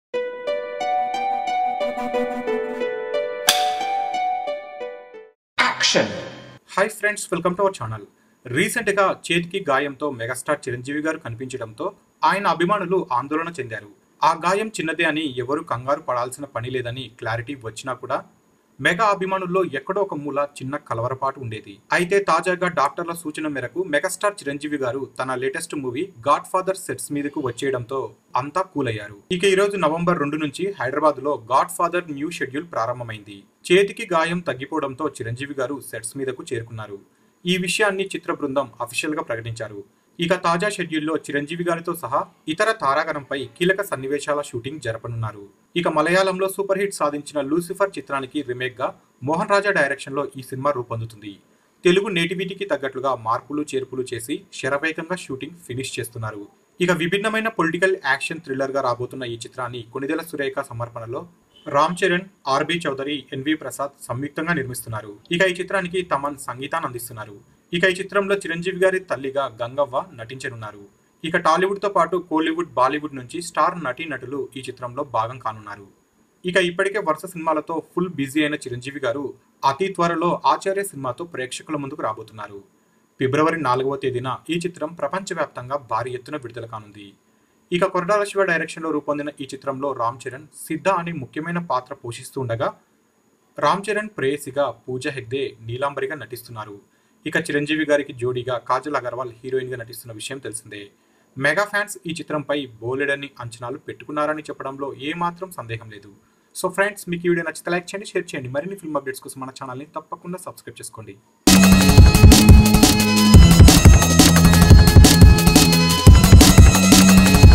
Megastar Chiranjeevi GodFather Movie Shooting Update | Get Ready
Megastar Chiranjeevi God Father Movie Shooting Update! Godfather is a Telugu action-drama movie directed by Mohan Raja. The film features Chiranjeevi in the lead role. Ram Charan, R B Choudary, N V Prasad are combinely producing the film under the banners of Konidela Production Company, Super Good Films respectively. Thaman S is likely to score the music for the film. Nirav Shah will crank the camera for the film.